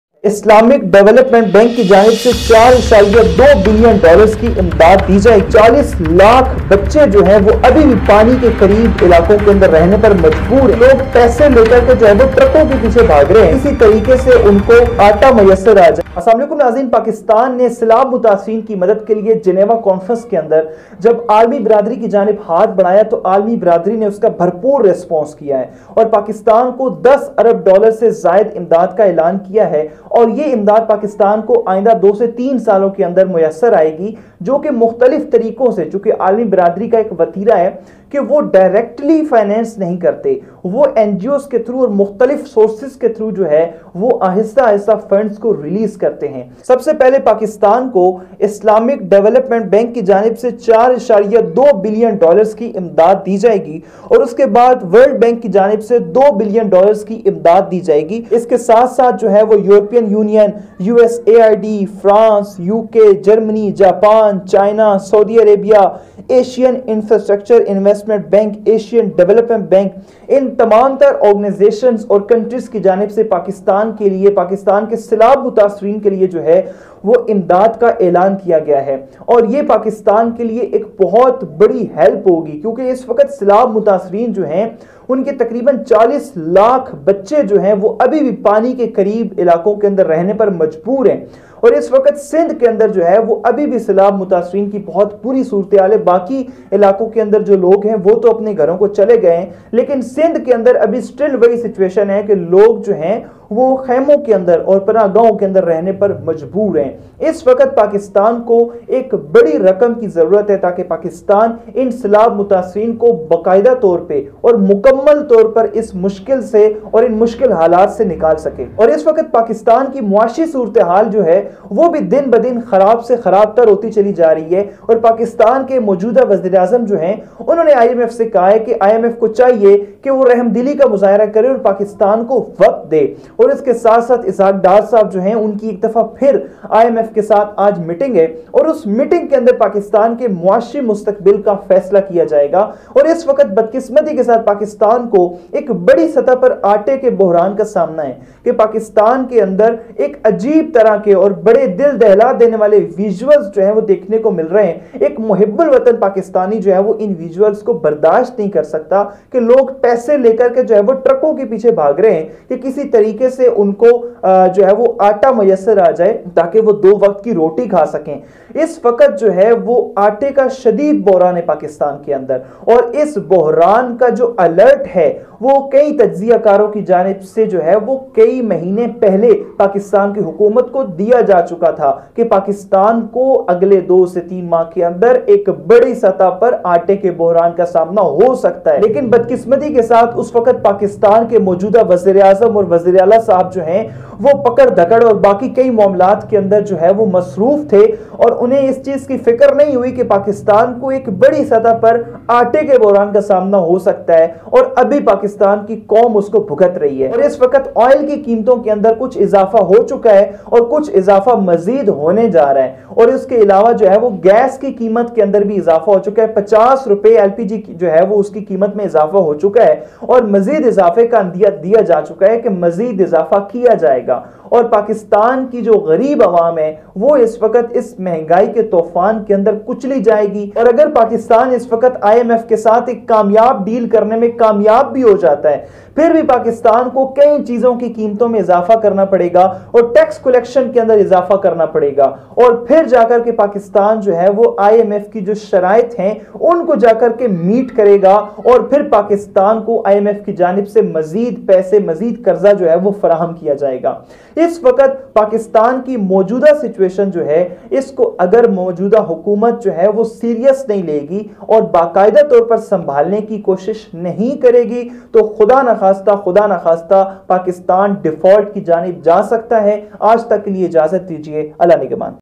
جنیوا کانفرنس میں سیلات متاثرین کے لیےعالمی برادری کے بڑے بڑے اعلانات | مہنگائ کی چکی میں عوام پس گئ
In this video, an analysis is being done on the current situation in the country, in which an overall view is being taken of the economy and other problems facing the country.